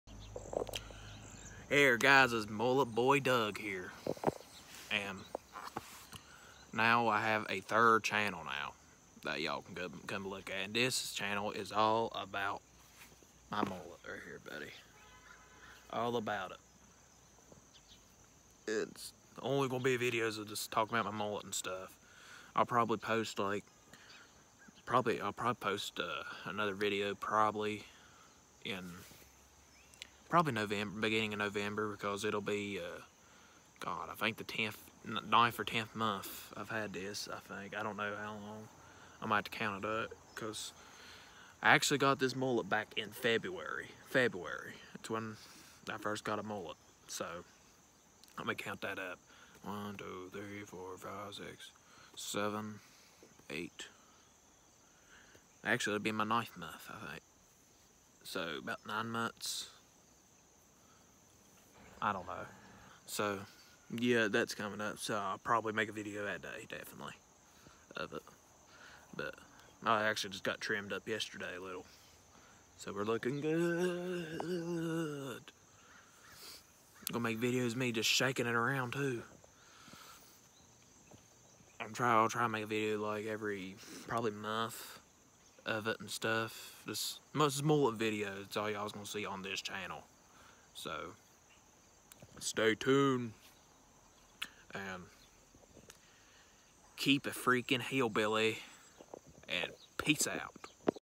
MY FIRST VIDEO!
Hope you all enjoy this channel. This channel is going to be me rocking my mullet and showing it off.🤠🤟🏻 YEE YEE!